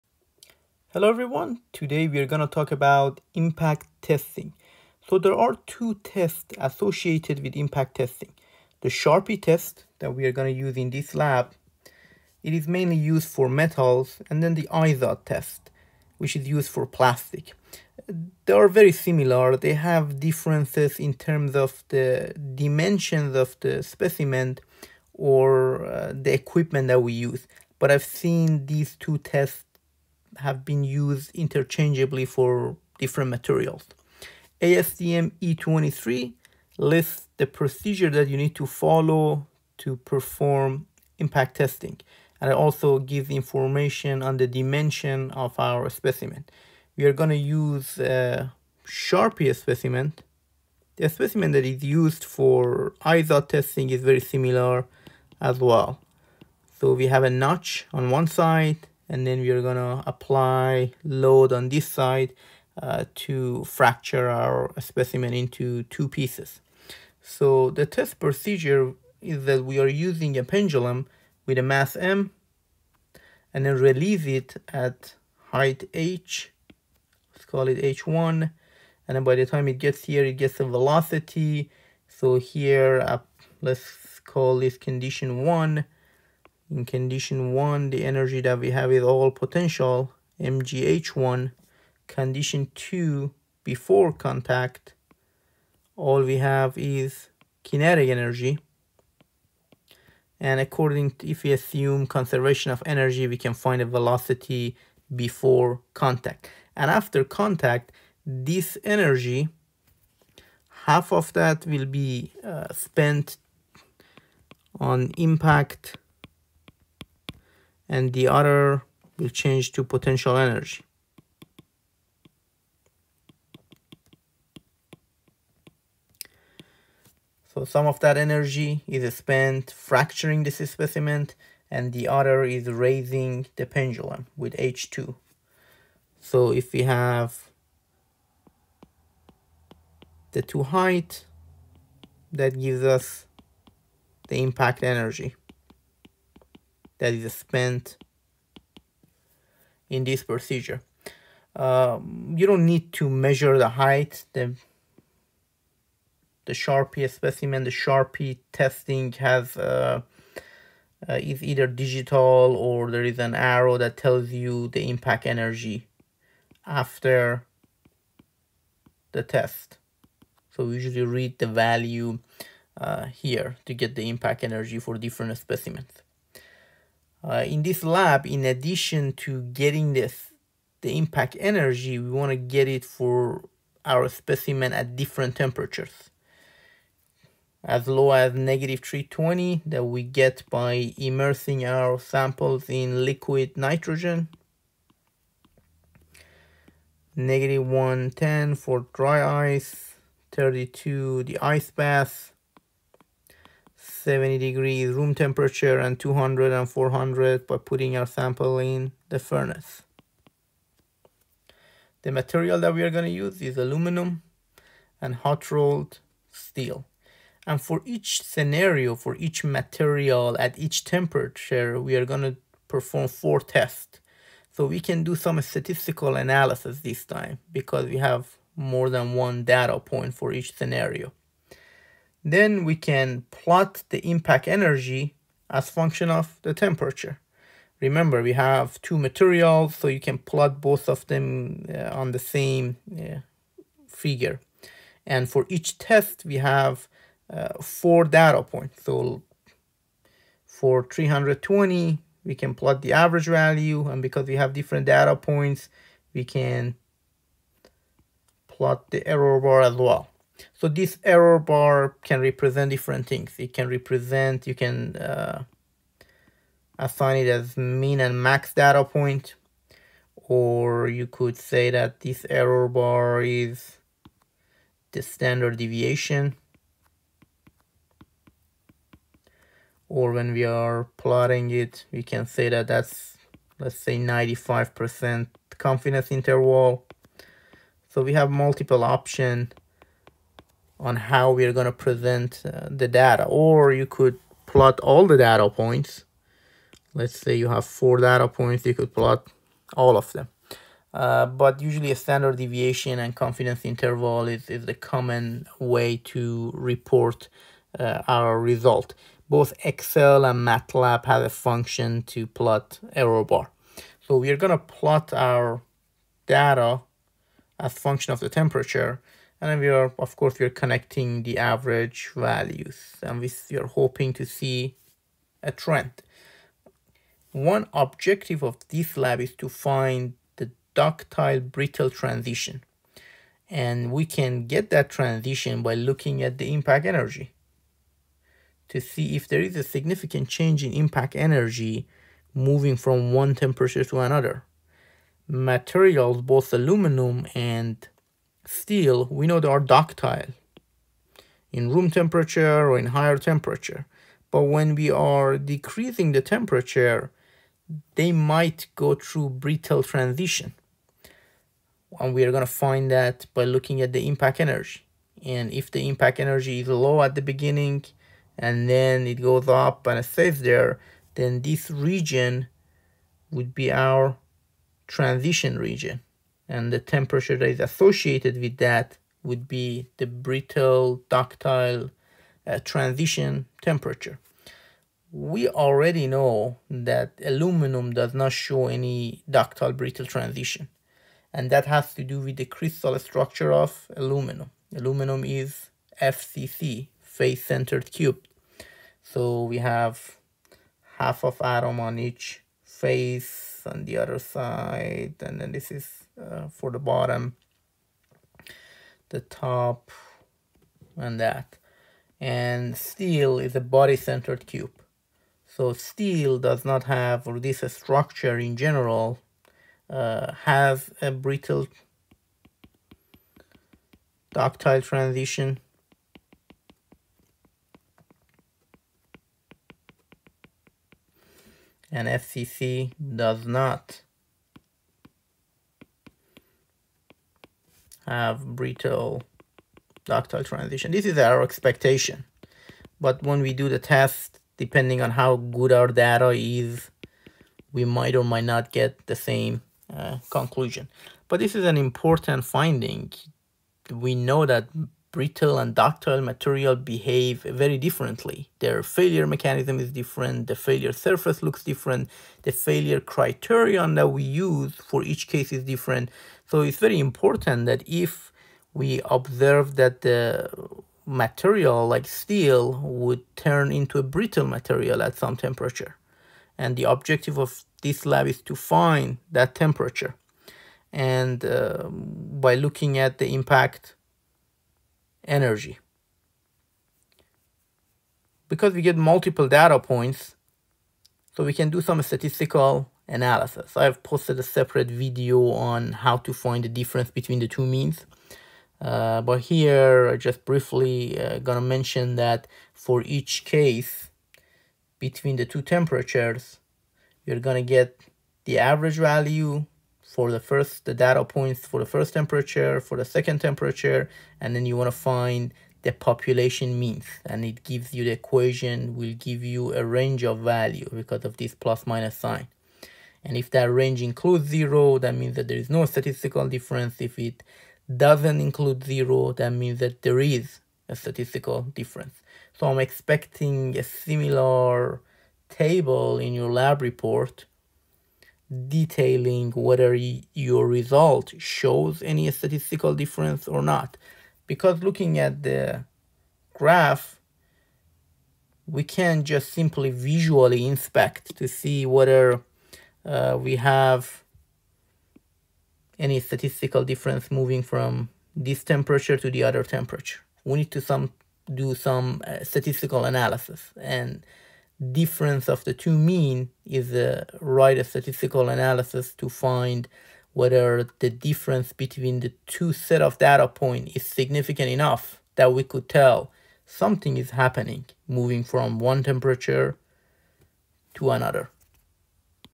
Impact Testing: Charpy Test to find the ductile-brittle transition
Charpy and Izod tests are conducted to determine the impact energy of different materials.
Ductile to brittle transition can be found by testing charpy specimen at different temperatures. Brittle material are expected to show lower impact energy than ductile material, therefore if we see a drop in the impact energy as the temperature decreases, we can identify the transition temperature. Material with face centered cubic (FCC) structures like aluminium do not have ductile to brittle transition, however, materials with body centered cubic (BCC) structure such as steel have ductile and brittle transition.